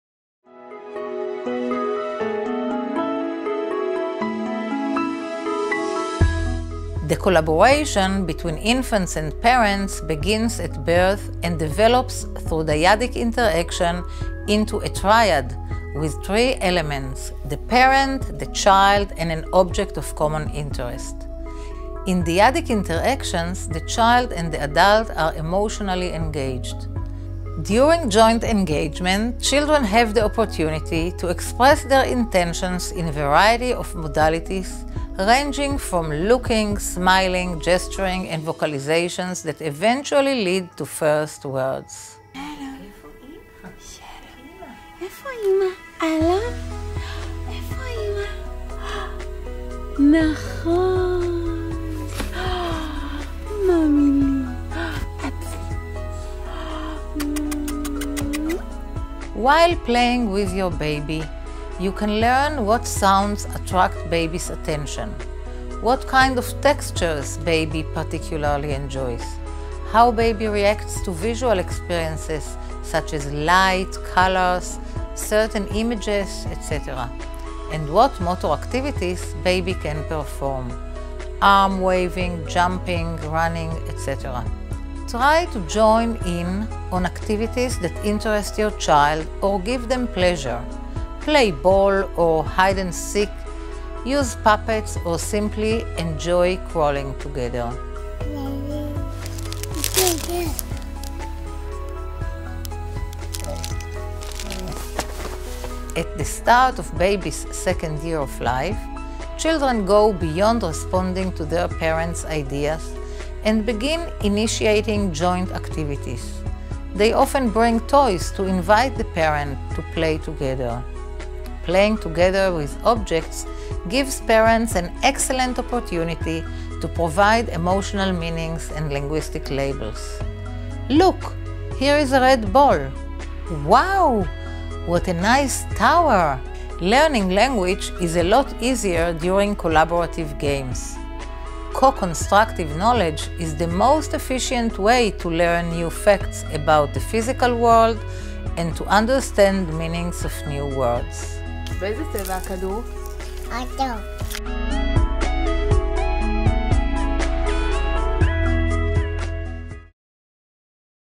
Expert Advice: Help Develop Your Baby's Speech through Play!
Watch this expert video to learn more about the complex process of baby's speech development, in this instance through play. Using various objects while playing with baby at various stages of his or her development, opens a whole new world of speech understanding and let's your child take the initiative and learn more as they grow.

Read full script to lean more about this fascinating baby development topic:
The collaboration between infants and parents begins at birth and develops through dyadic interaction into a triad with three elements: the parent, the child and an object of common interest. In dyadic interactions the child and the adult are emotionally engaged.  During joint engagement children have the opportunity to express their intentions in a variety of modalities ranging from looking, smiling, gesturing and vocalizations that eventually lead to first words.
While playing with your baby, you can learn what sounds attract baby’s attention; what kind of textures baby particularly enjoys; how baby reacts to visual experiences such as light, colors, certain images, etc.; and what  motor activities baby can perform: arm-waving, jumping, running, etc..

Try to join in on activities that interest your child or give them pleasure: play ball or hide-n-seek, use puppets or simply enjoy crawling together.

 At the start of baby’s second year of life, children go beyond responding to their parents’ ideas and begin initiating joint activities. They often bring toys to invite the parent to play together. Playing together with objects gives parents an excellent opportunity to provide emotional meanings and linguistic labels. "Look, here is a red ball", "Wow!!! What a nice tower."
Learning language is a lot easier during collaborative games. Co-constructive knowledge is the most efficient way to learn new facts about the physical world and to understand meanings of new words.